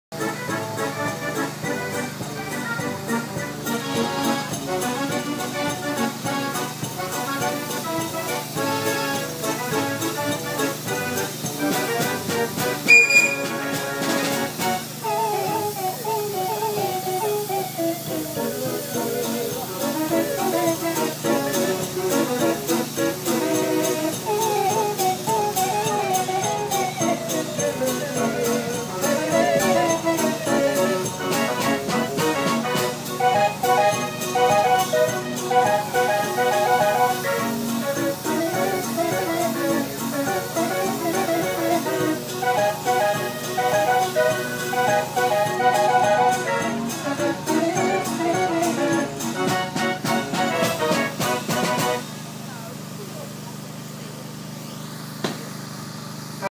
1) Lytham Hall Steam Rally/Fair - Sunday 16th Sept 2018
Lytham Steam Fair - Sunday 17 September 2017 organised by Lytham Rotary Club. Steam engines, stationary engines, classic cars, classic motorcycles, fun fair and donkey rides. Fairground organ /  Orchestrion / music machine owned by Philip Cowell & Sons of Grimsargh, near Preston Lancs.